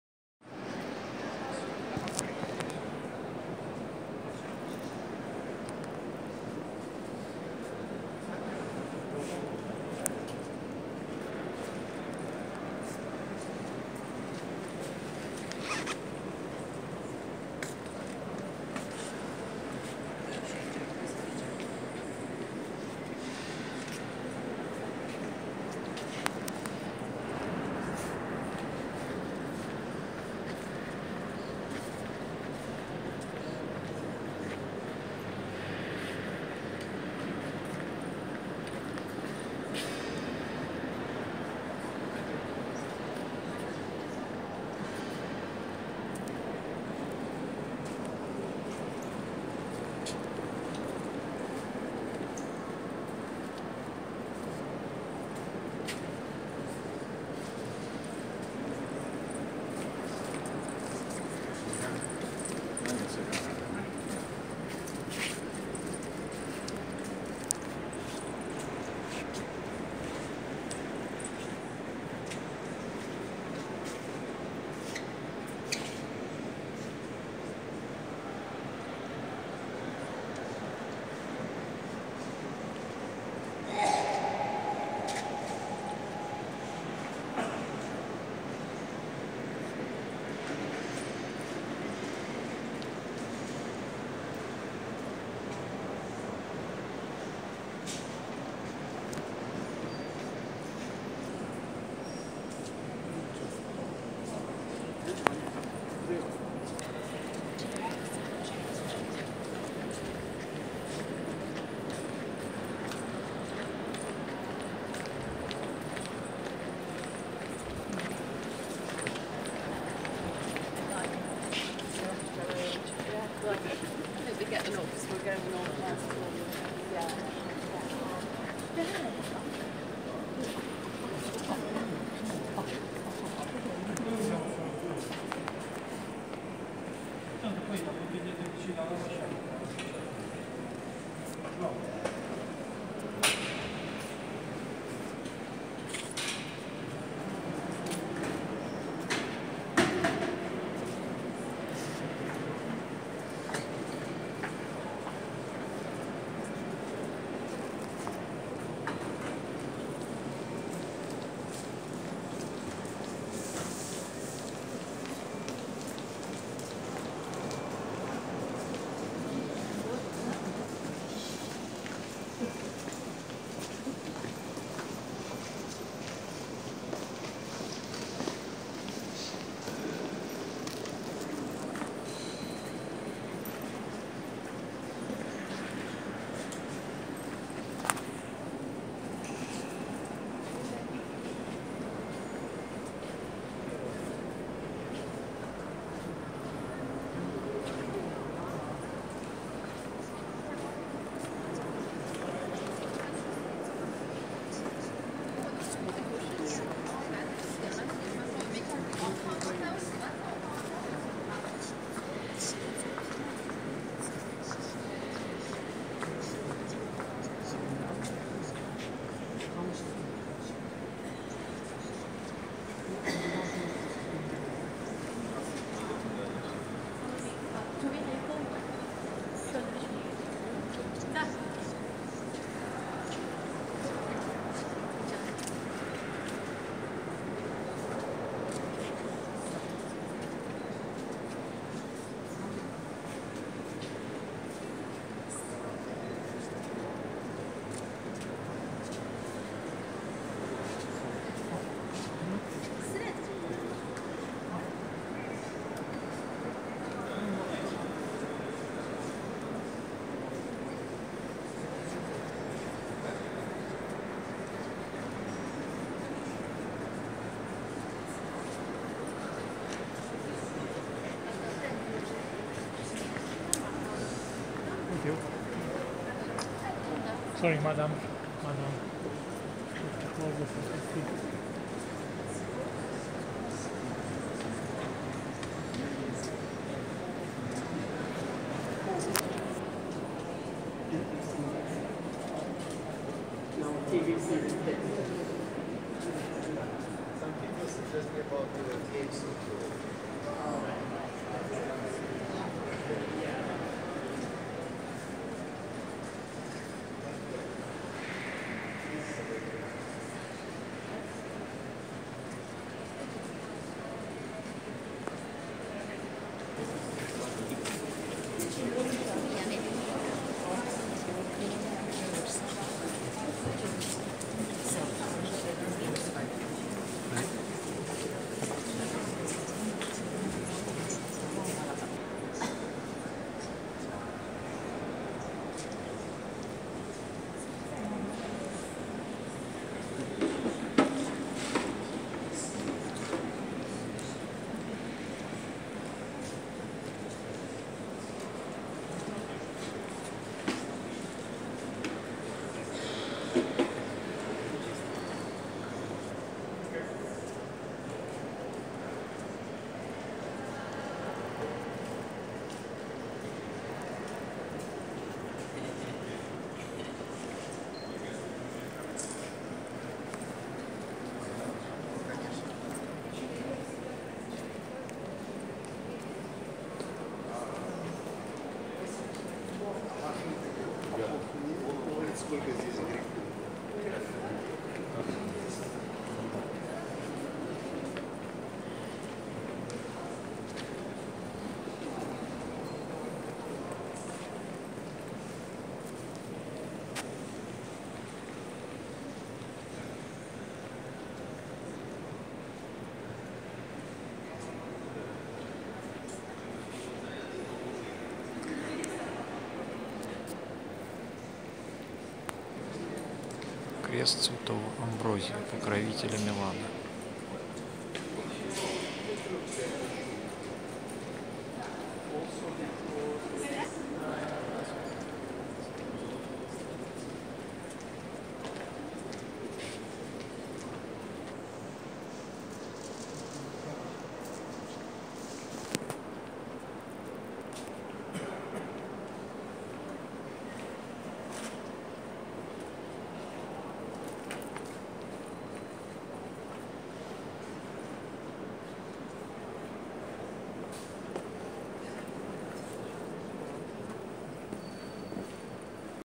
Candles & sculptures of Milano cathedral
Candles, archs, ceilings & sculptures of Milano cathedral (Milano Duomo).